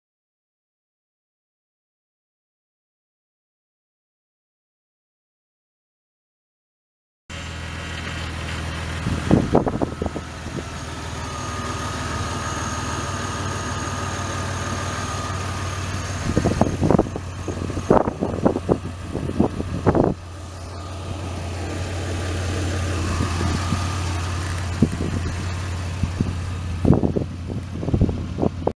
2006 Chevrolet Impala LT for sale | no-reserve Internet auction March 20, 2018
Government Auction
View a detailed listing of this item and place bids

2006 Chevrolet Impala LT
125,598 miles on odometer
3.5L V6 OHV 12V FFV gas engine
Automatic transmission
AC and heat
Tilt
Cruise
AM/FM/CD
Power windows, locks, mirrors, and seat
Cloth interior
225/60R16 tires
Wiring harness has been repaired
Kansas title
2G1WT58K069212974
It's located in Ulysses, KS, and we're selling it to the highest bidder regardless of price.